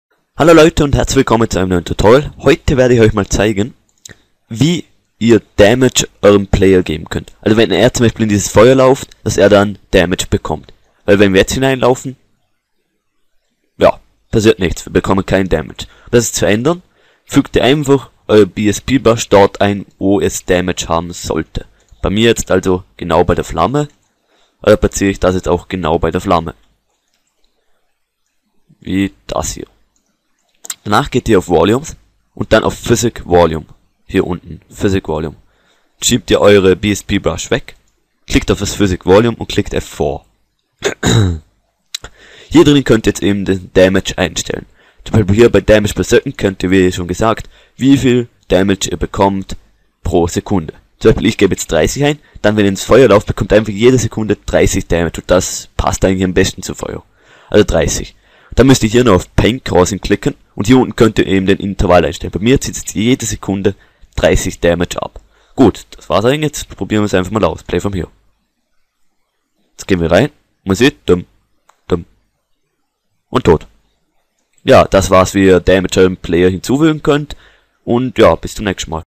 UDK Tutorial: give player damage (german)
Wie man macht, dass der Spieler schaden bekommt wenn er in diesen Bereich kommt